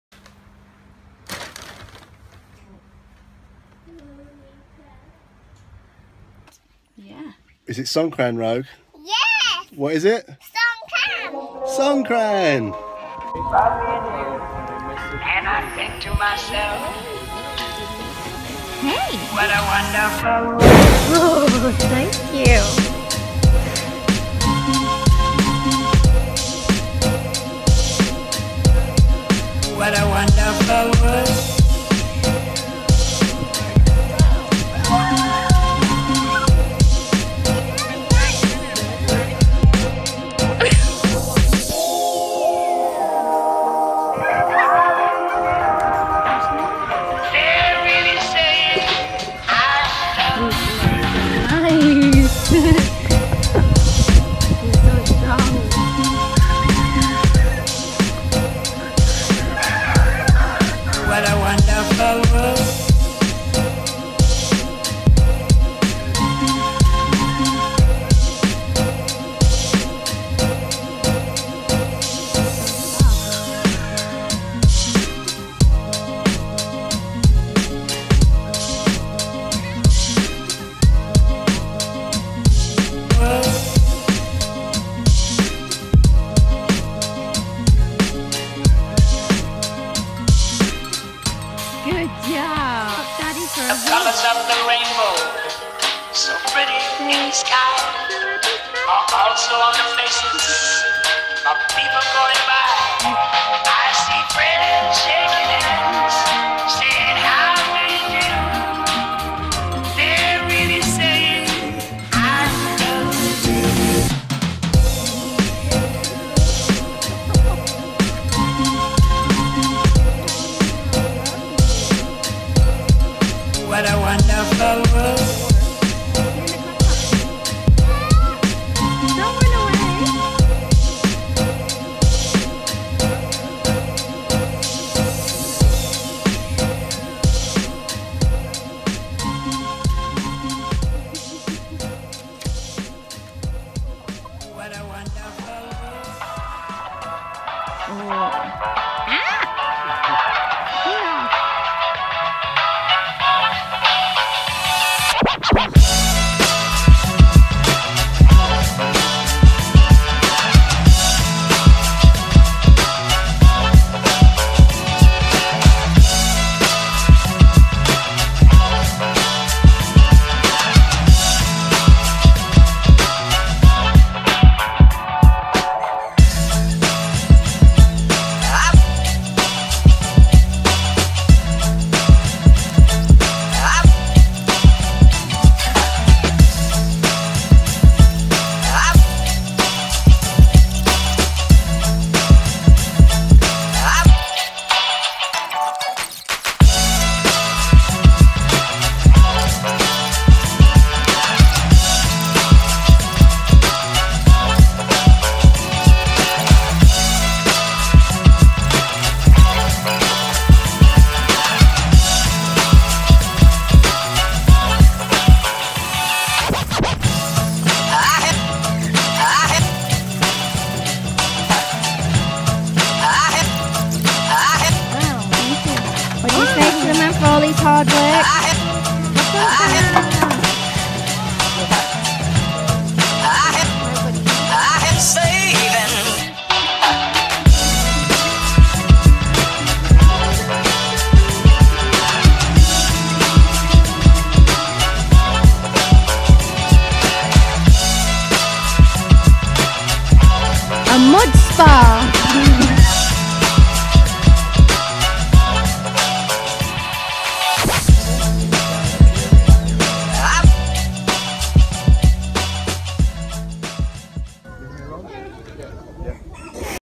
Ginger Farm CHIANG MAI - Songkran water festival day1 - THAILAND vegan
day 1 of Songkran we went to ginger farm